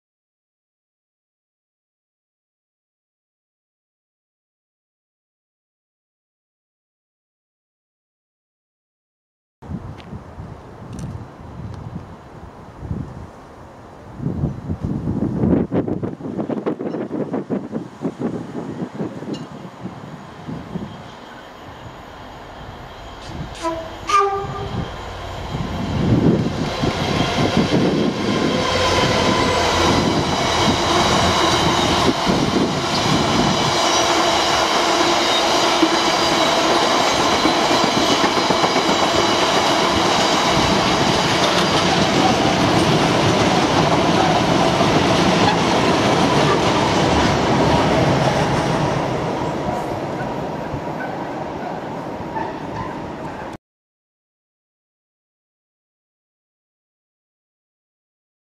FL 66623 Working 6M92 West Thurrock Sidings to Hope (Earles Sidings) Fhh Passing Barking 12/09/2015
FL 66623 Working 6M92 West Thurrock Sidings Fhh to Hope (Earles Sidings) Fhh Passing Barking 12/09/2015